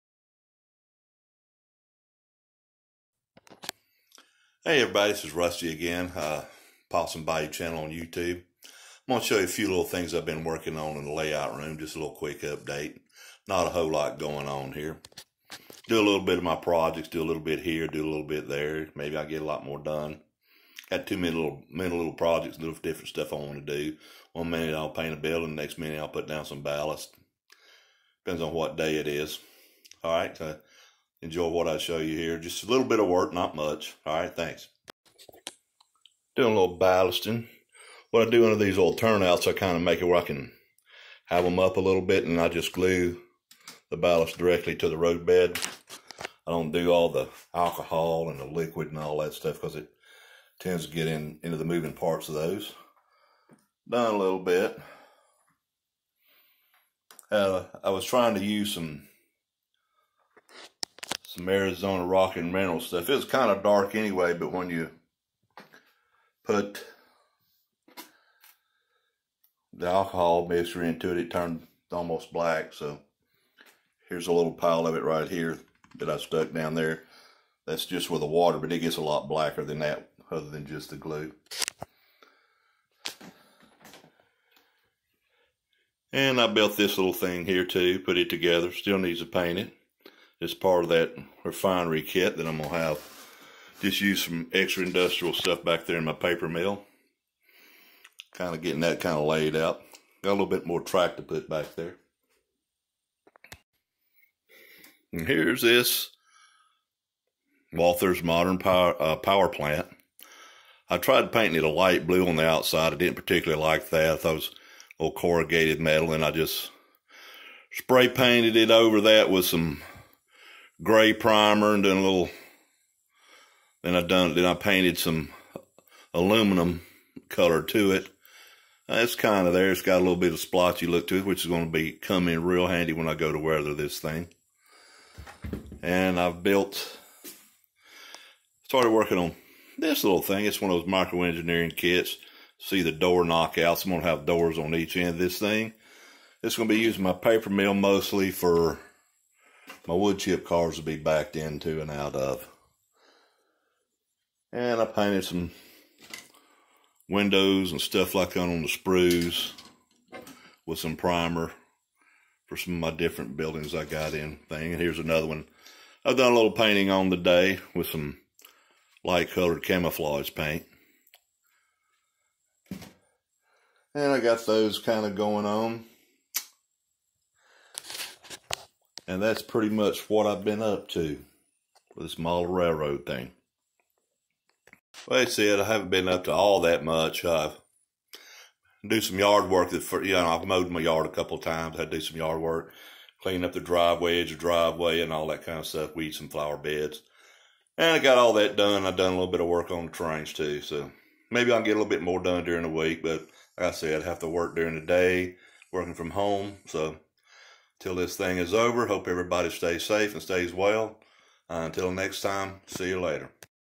video 6
Just a little update . A little train work during the show stopper